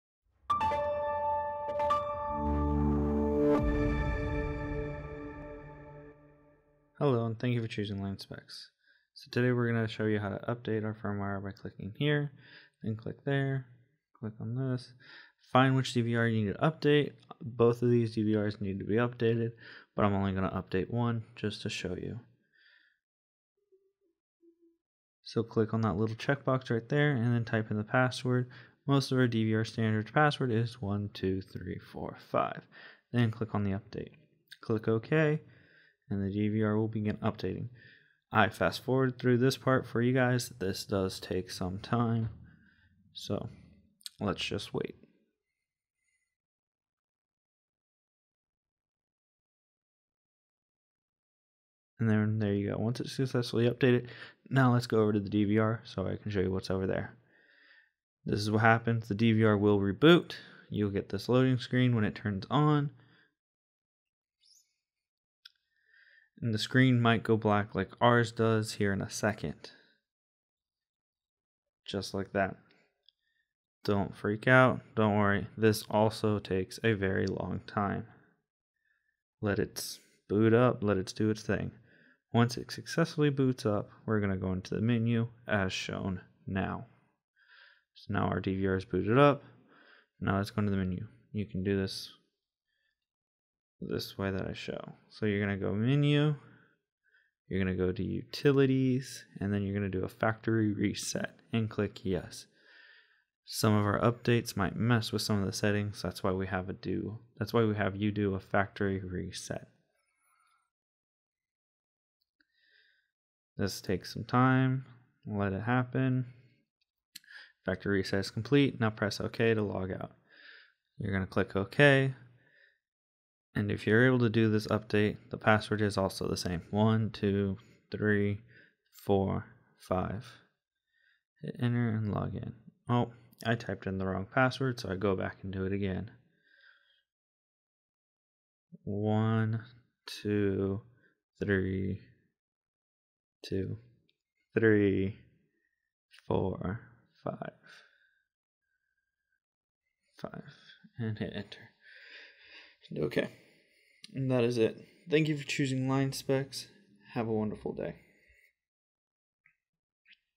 DVR Firmware Update
How to update the DVR Firmware using the Windows Firmware Updater.